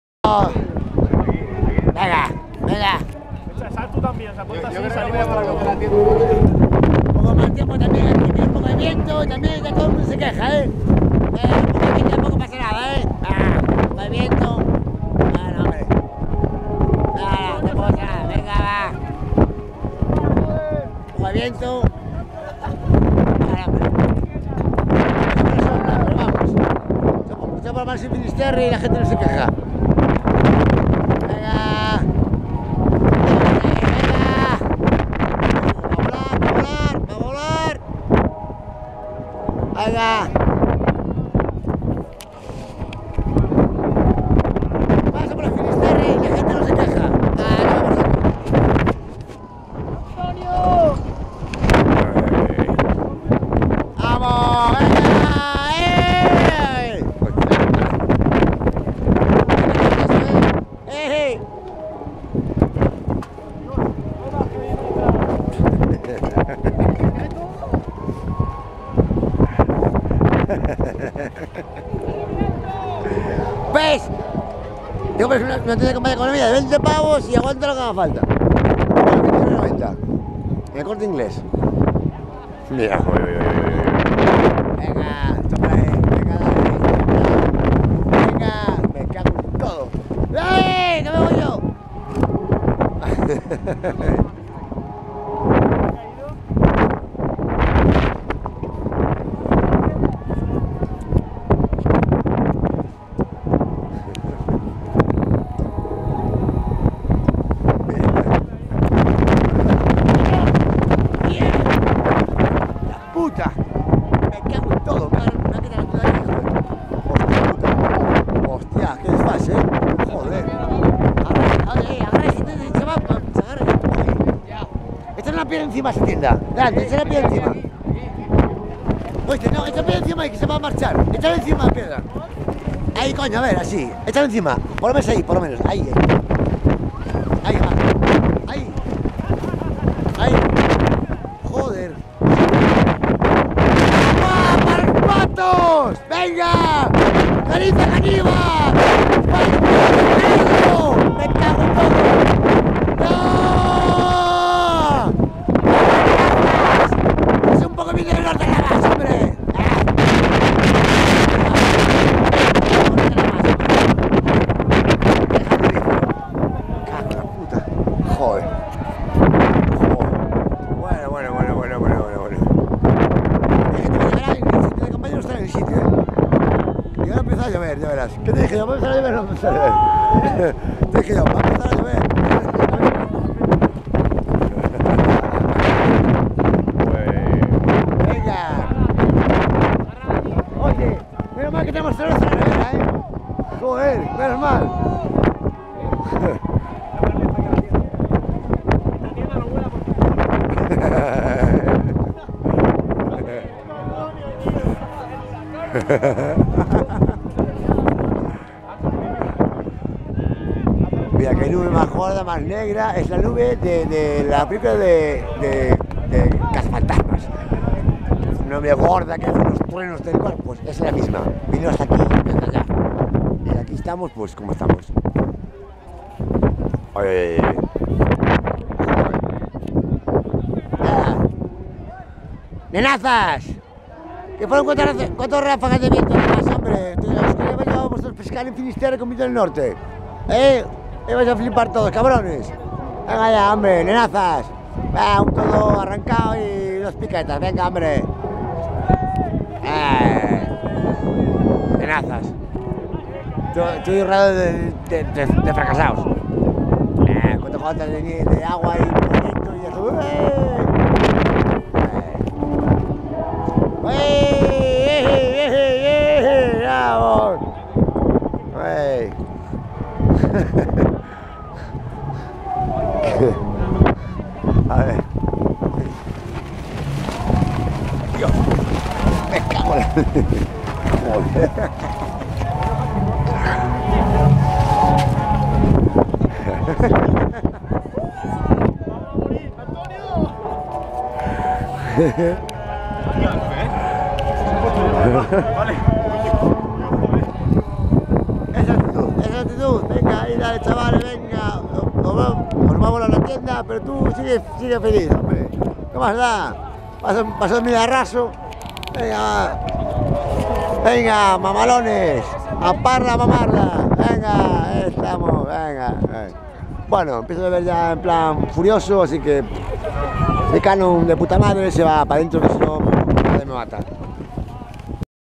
Tornado en leyendad el rock 2015,1ª parte.Restransite:Carlitros.
Atencion a Joaquín tomándose una todo pancho en medio del katrina del leyendas!Angel,ni siente,ni compadece,ajjajajaj,todo tranquilo que aqui,no pasa nada!